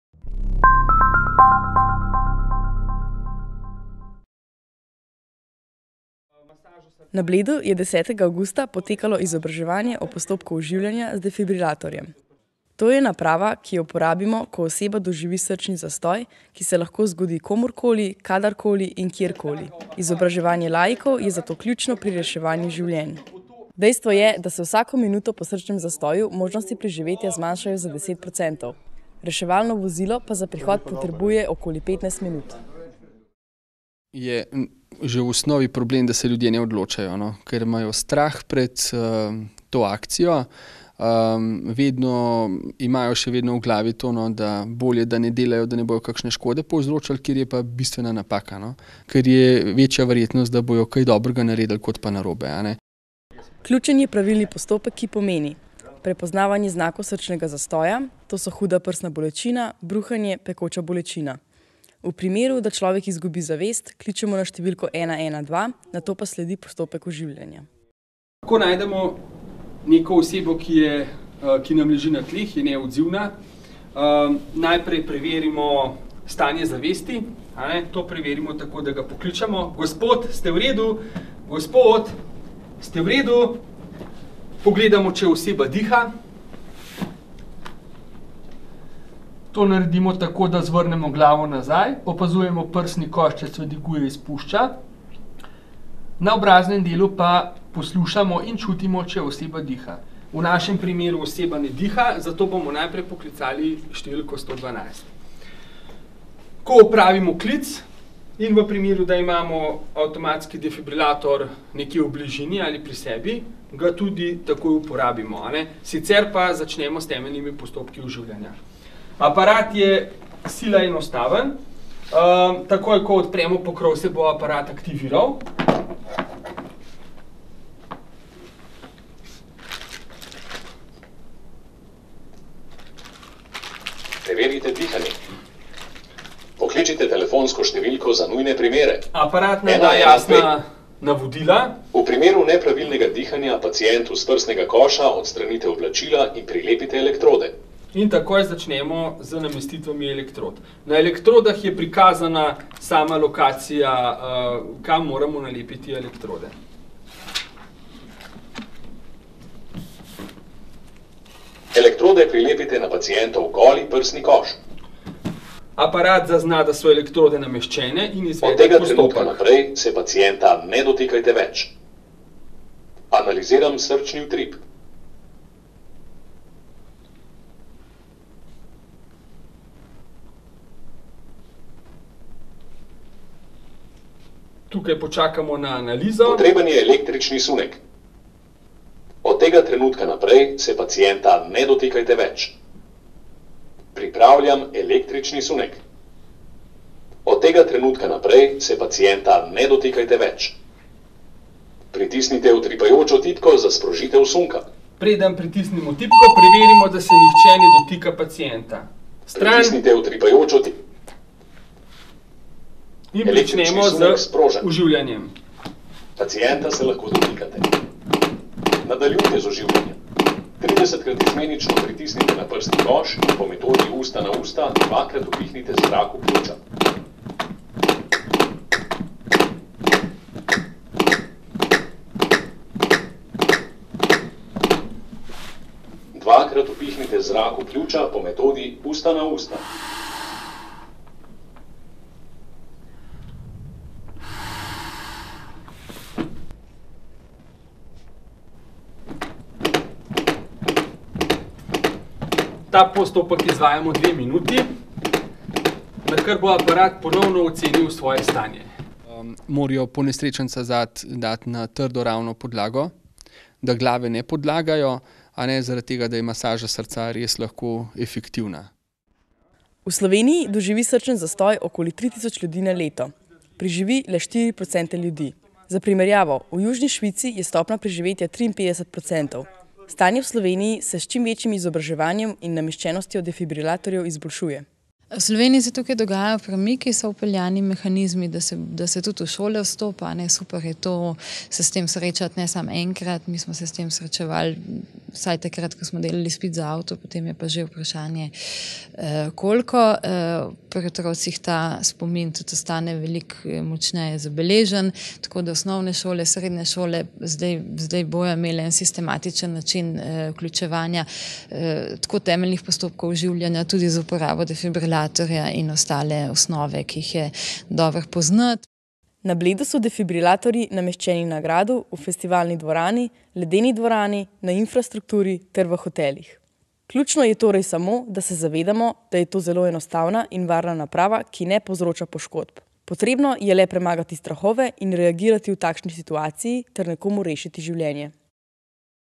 Pomagajmo, vsaka sekunda šteje!
Veriga preživetja, postopek, ki v primeru srčnega zastoja rešuje
življenje, pomeni prepoznavanje znakov srčnega zastoja, klic na 112,
postopek oživljanja, defibrilacijo in dodatne postopke oživljanja.
Omenjeni postopki so velikokrat v veliki meri odvisni od laikov,
očividcev, ki do prihoda reševalne ekipe lahko rešijo življenje. Kako
uporabljati defibrilator in kje na Bledu so defibrilatorji  javno
dostopni v prispevku Pomagajmo, vsaka sekunda šteje!

Novinarka: Eva Bremec
Snemalci: Tadej Pernuš, Jon Razinger
Montaža: Tadej Pernuš